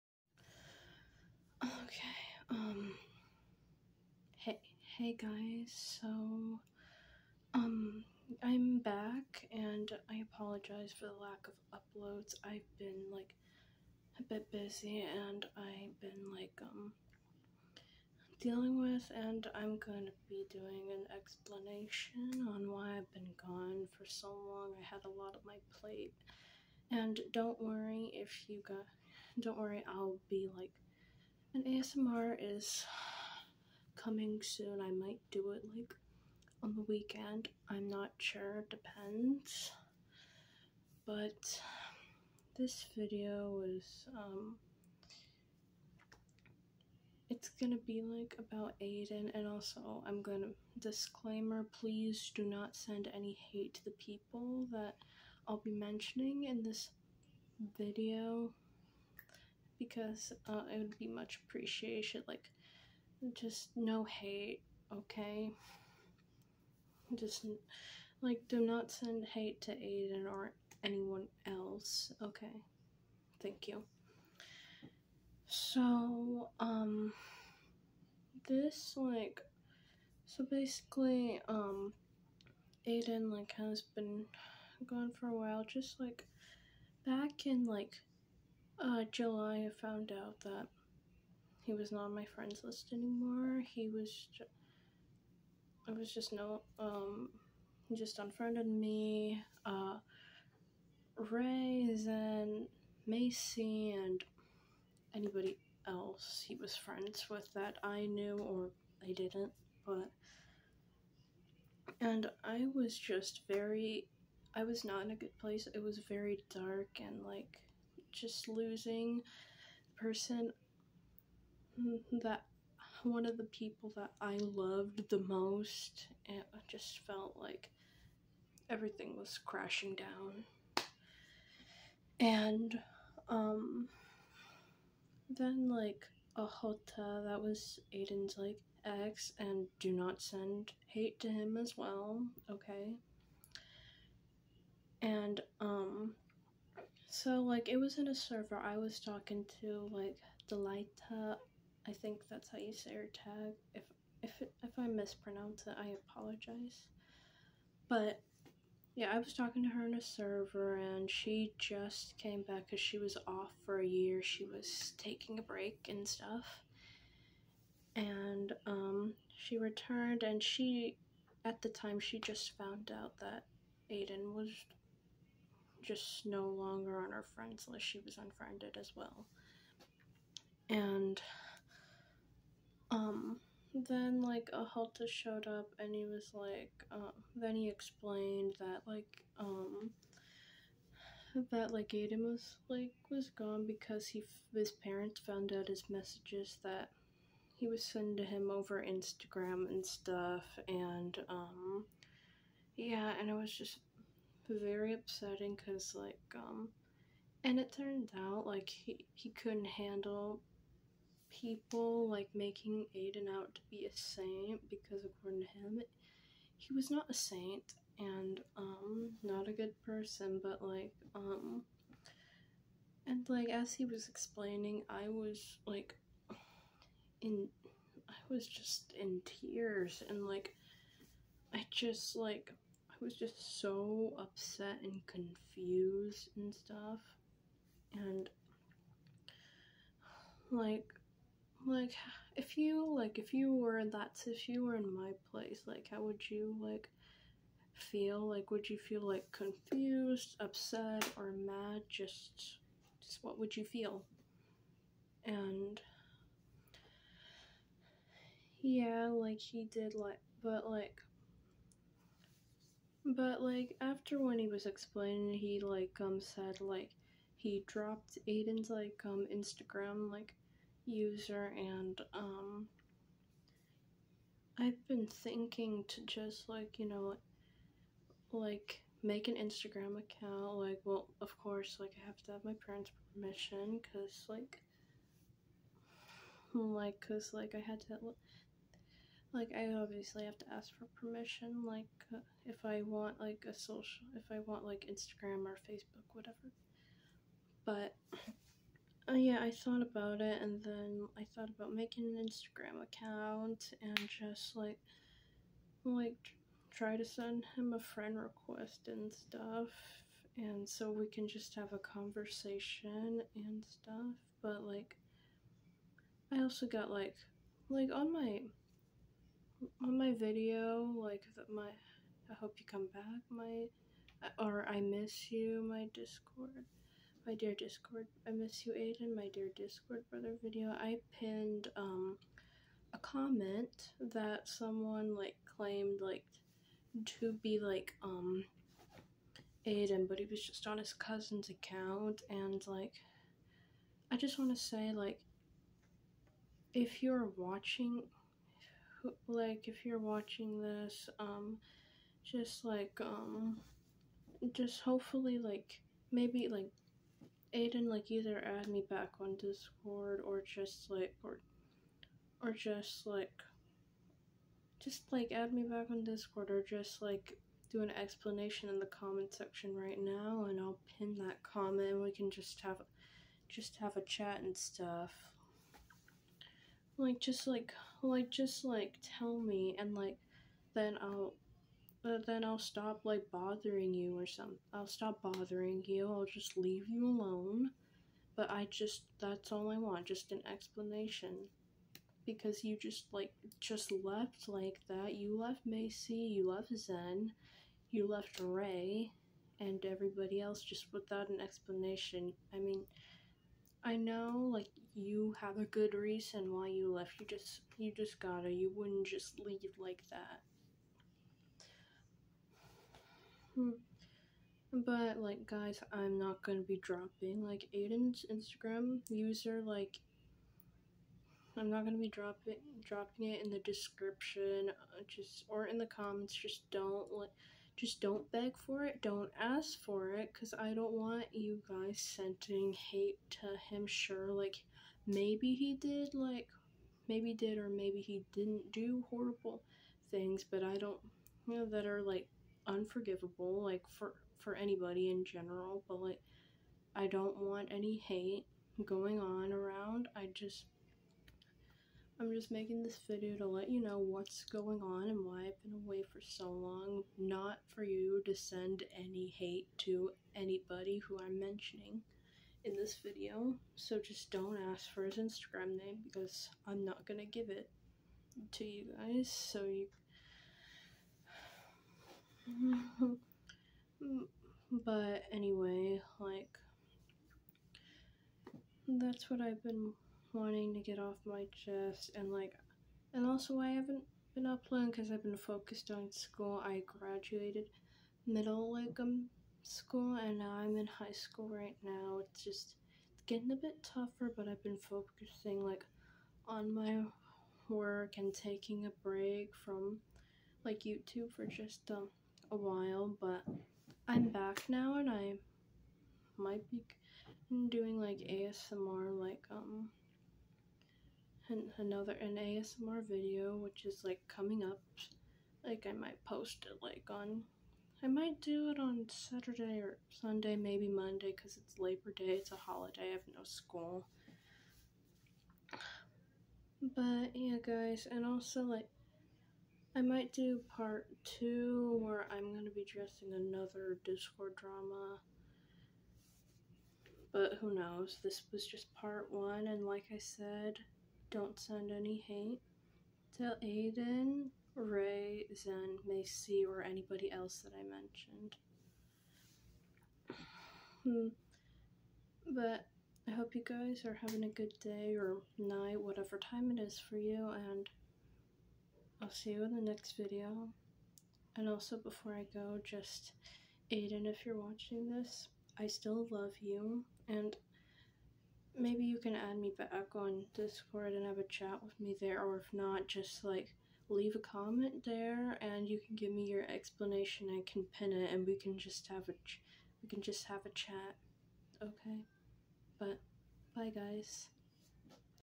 An explanation on what's been going on part 1?
Hey guys hope you liked the video sorry for the lack of uploads but an ASMR video is coming up but please don't send any hate to anyone I mentioned and don't ask for Aidens Instagram name even if your not going to send him any hate I still don't want to give it but I love you guys see you in the next video ^^